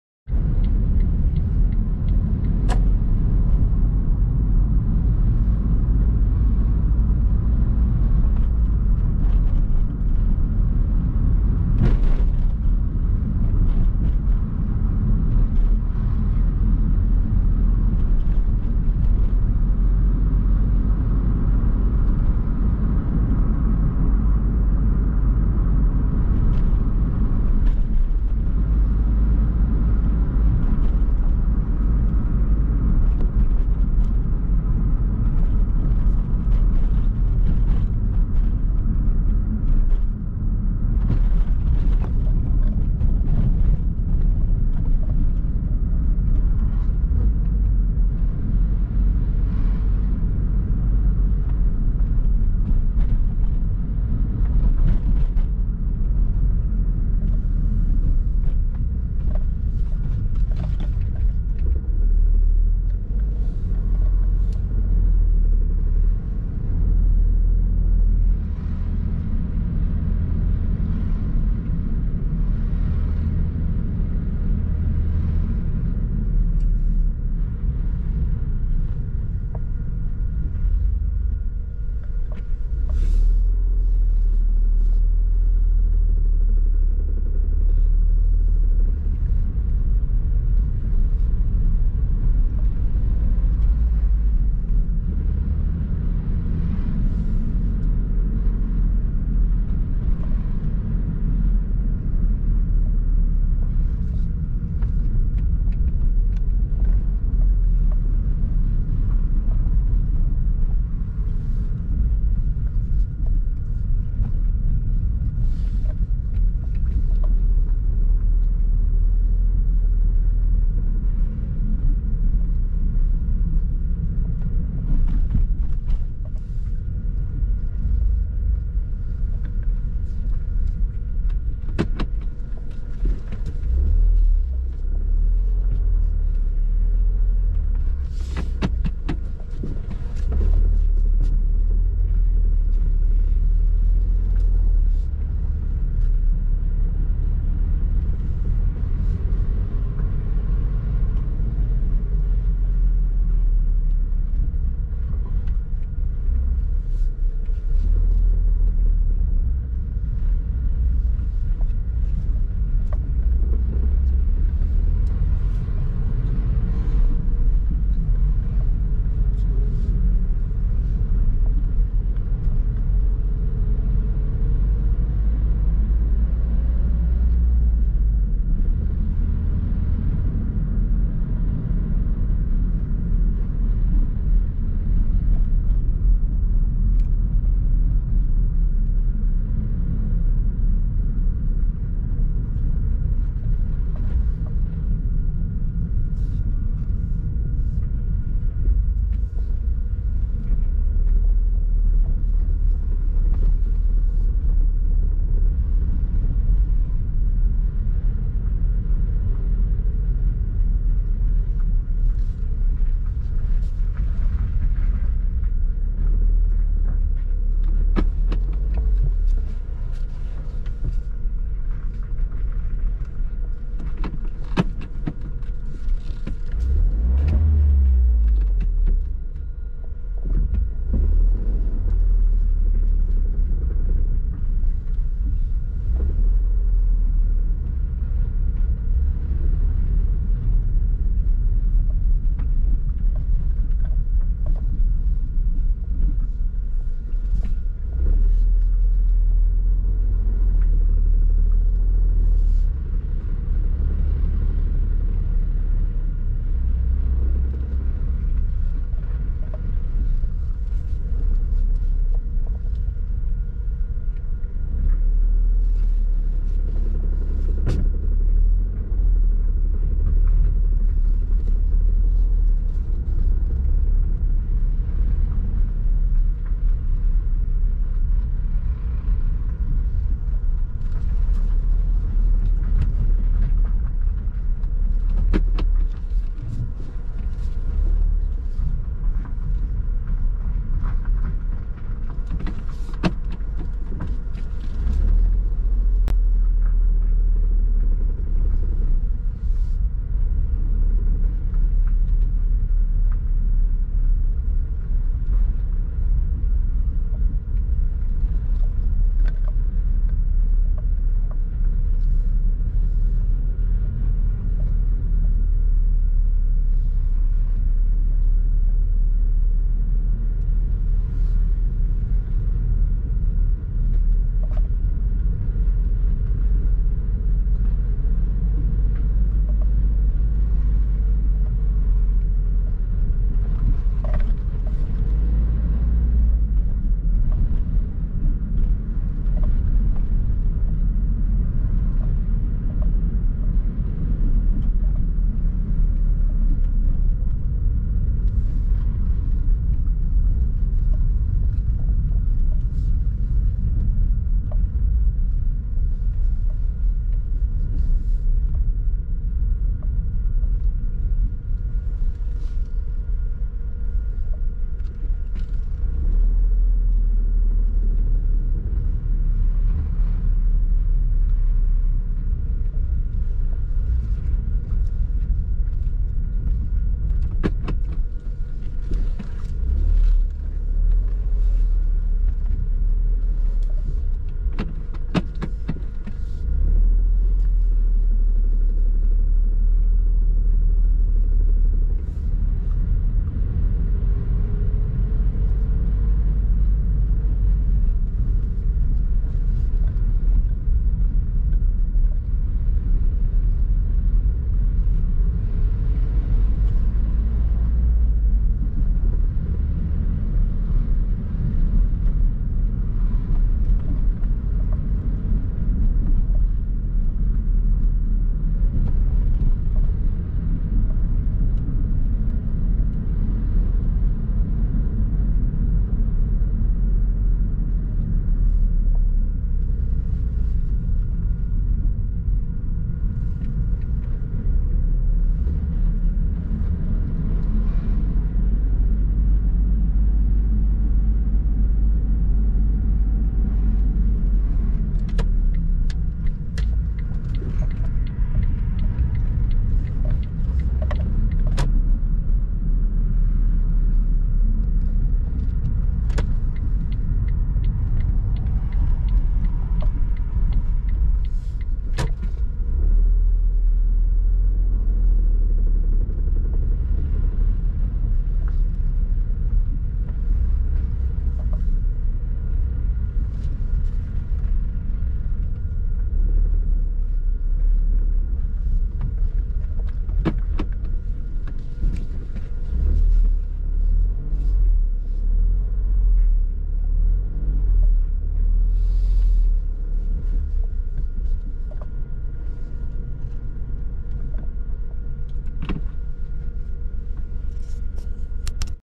RUN DOWN Birmingham Hood...Guest Grove in NewTown B19, 9BOYZ
A DRIVE THROUGH THE GUEST GROVE ESTATE IN BIRMINGHAM TOWN NEWTOWN IN B19. HOME TO DRILL RAPPERS TRILLS, NLOOSE AND THE 9BOYZ

New video posted every SUNDAY